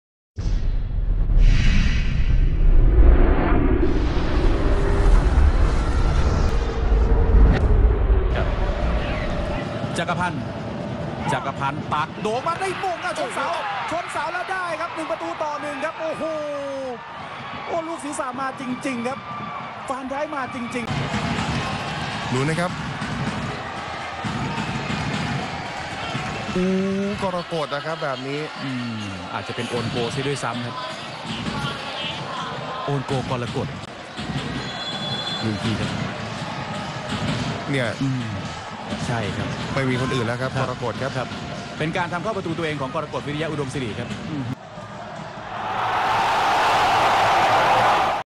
Carmelo González - Chonburi FC 2-2 Suphanburi FC - TPL 2015
Carmelo González scores for Suphanburi FC in their 2-2 away draw wth Chonburi FC on 27 September 2015 in the Thai Premier League.